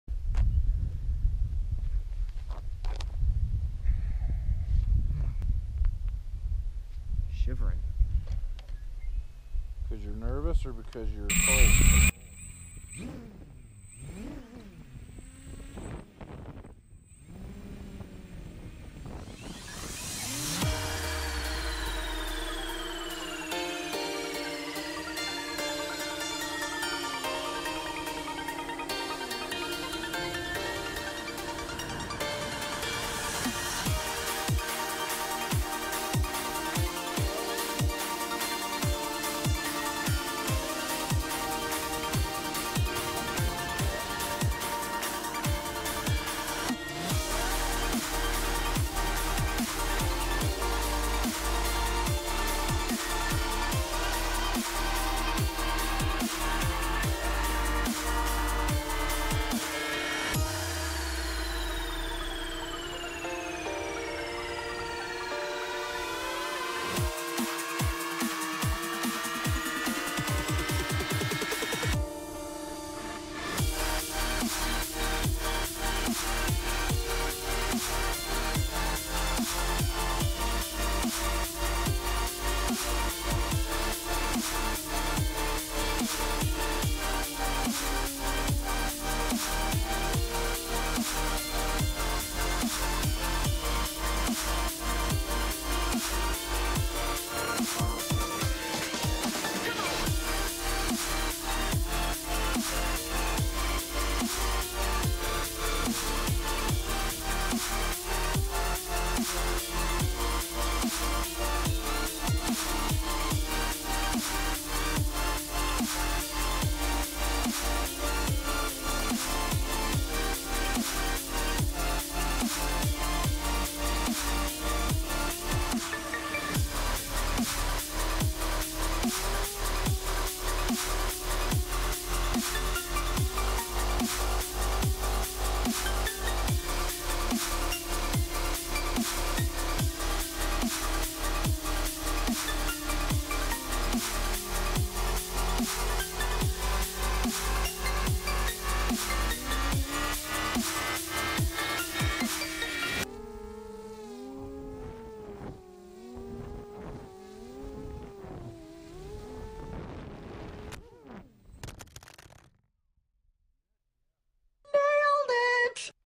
My first ever quadcopter flight
went out to the track today and flew my quadcopter and had a blast! hope you enjoy!